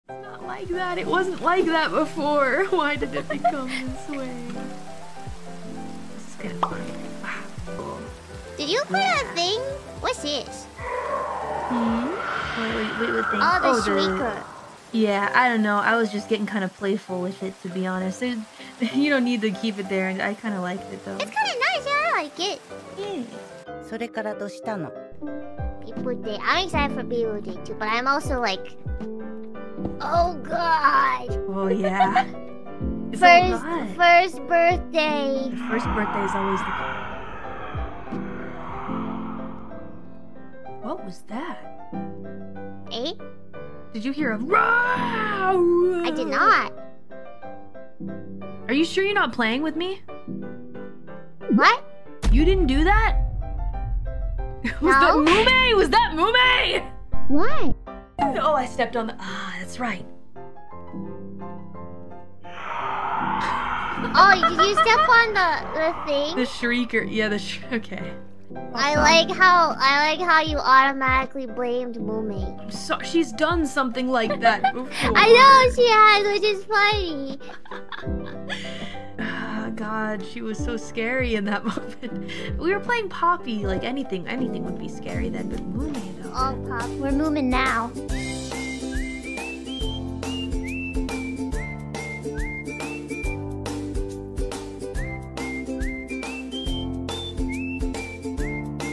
[ENG SUB/Hololive] Calli is still haunted by Mumei's prank and has developed PTSD
Credits :
BGM